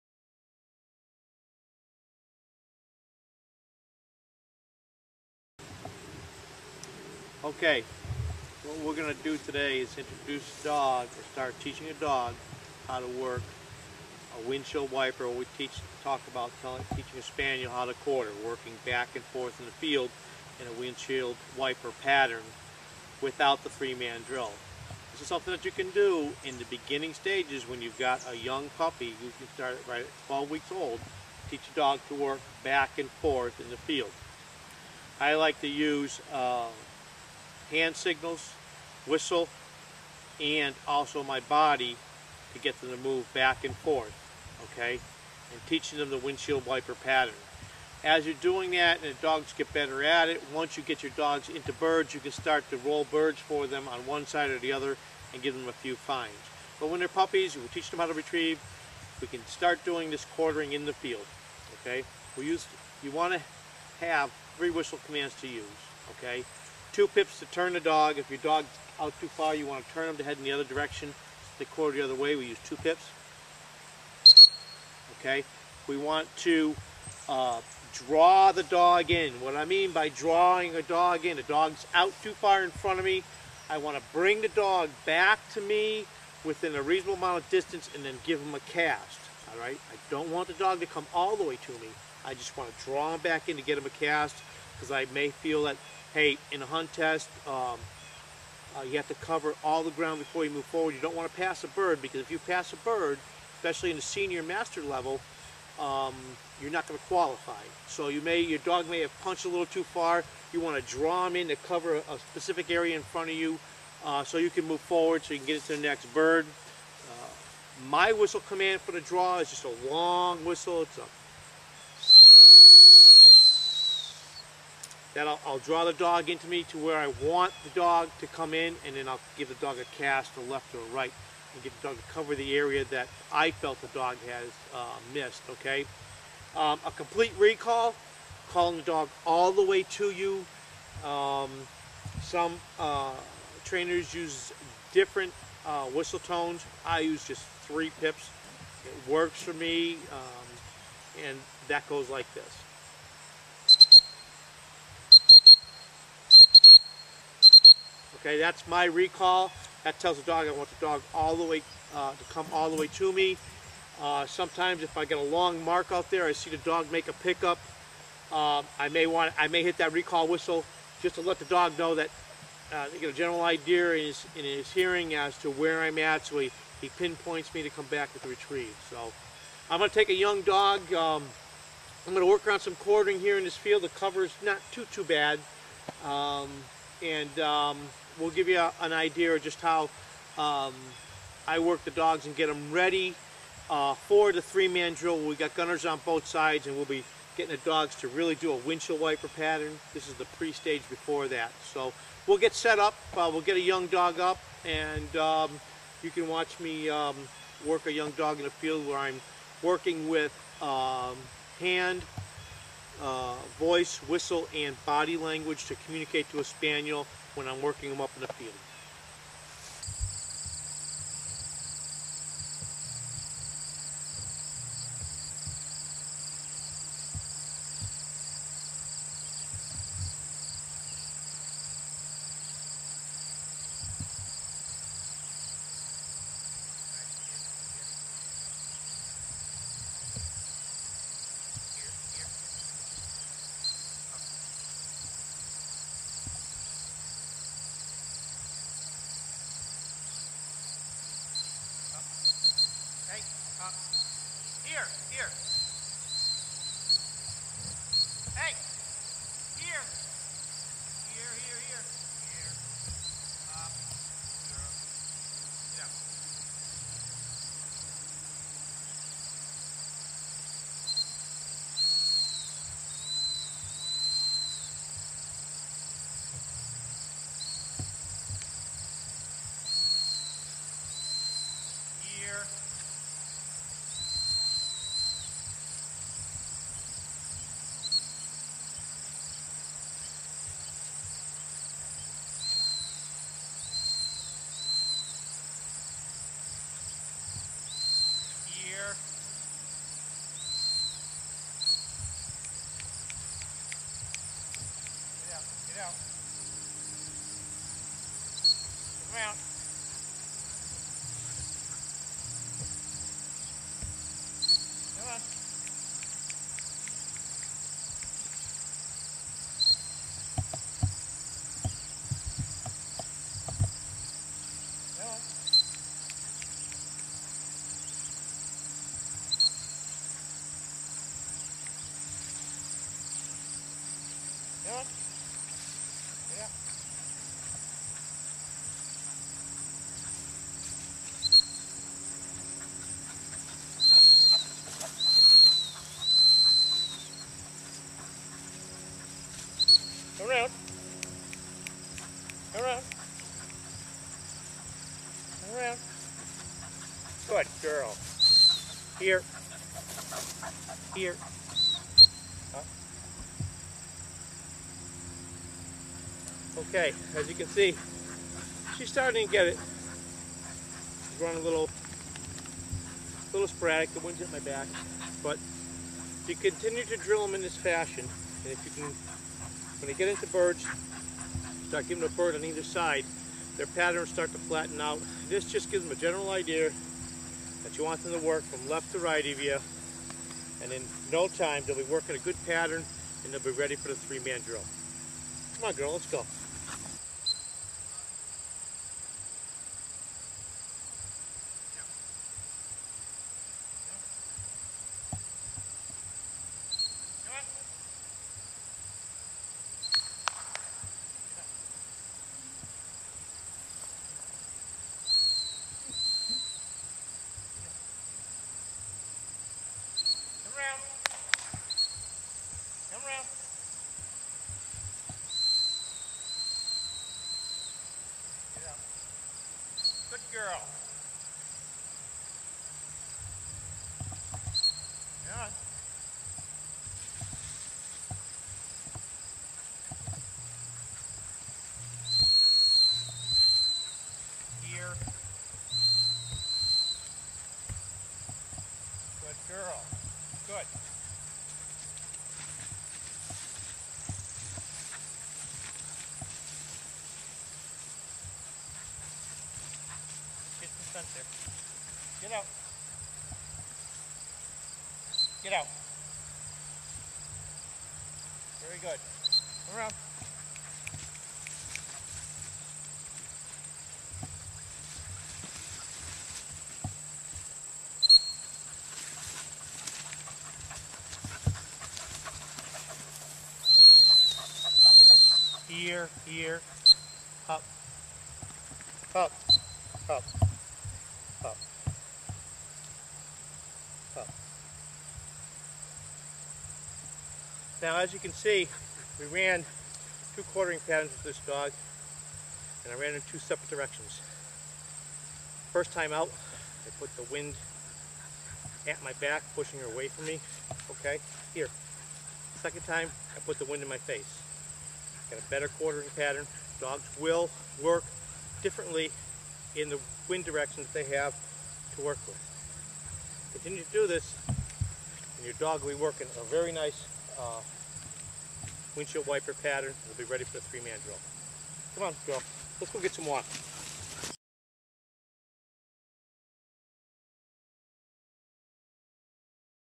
Gundog Training: Teaching a Springer Spaniel to Quarter a Hunting Pattern
early stages of pattern work for a springer spaniel